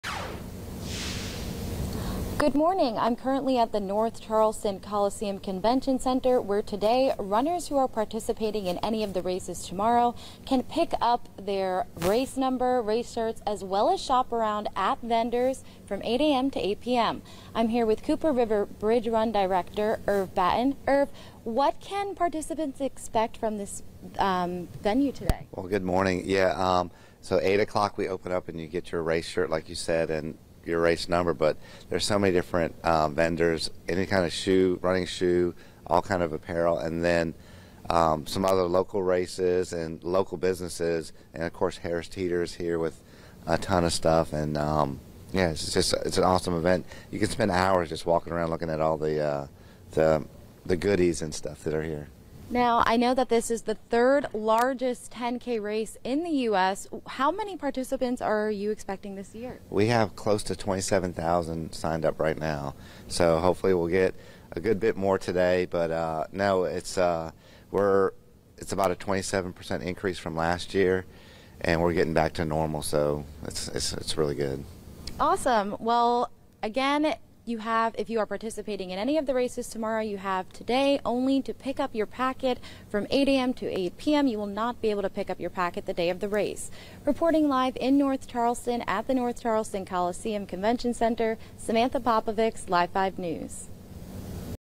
VIDEO: 46th Cooper Bridge Run Expo continues Friday
After picking up race gear, the runners can visit nearly 200 vendor booths with free giveaways, nutrition information and fitness demonstrations.